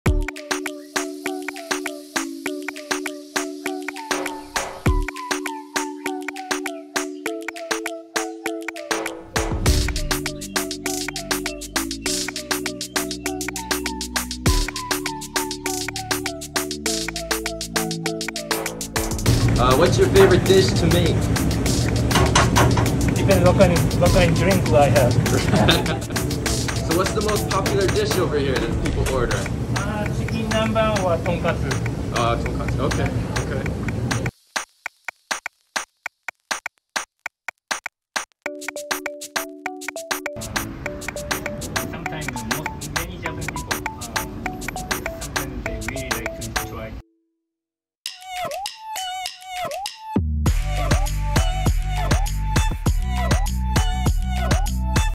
@Sho's Kitchen | Behind The Scenes
Sho’s Kitchen offers reasonably-priced Japanese food such as Tonkatsu, Chicken Nanban, Tempura Rolls to name a few. Prices starting from $10. He uses local ingredients as much as possible to support local farmers and sustainability